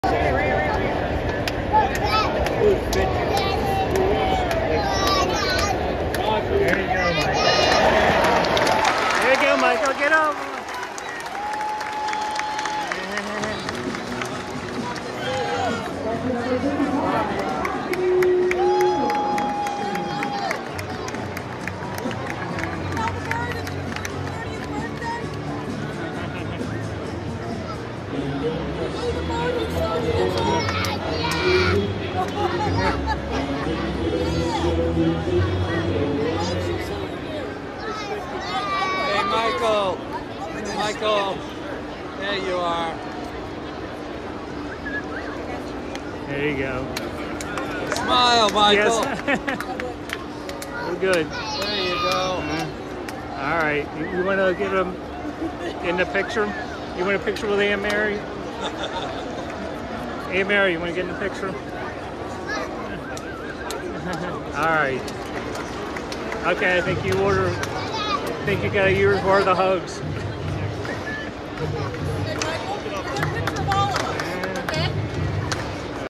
Meet the Oriole Bird
Oriole Bird came to our seats to meet Michael at Orioles Park at Camden Yards